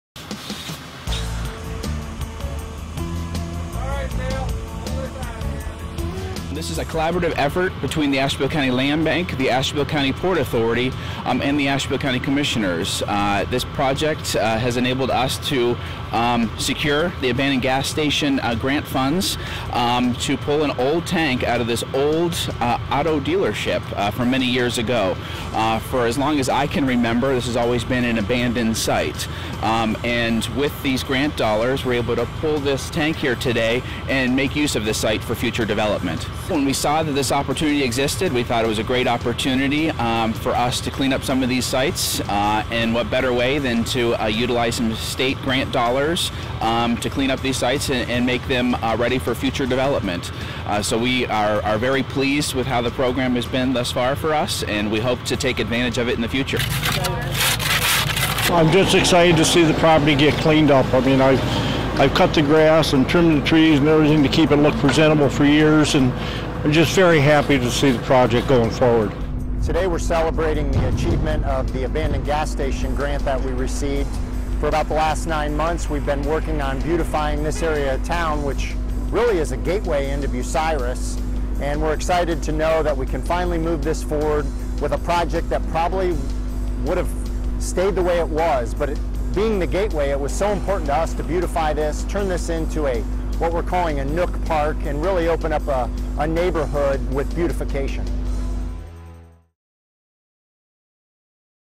Cleaning up Abandoned Gas Stations
The Ohio Development Services Agency presented to more than 200 economic development professionals at OEDA 360 in Dublin, Ohio. This event provided information on how participants can leverage state resources to capitalize on their hometown advantage. Communities across Ohio are cleaning up abandoned gas stations with help from the state. The cleaned up sites are ready for new development.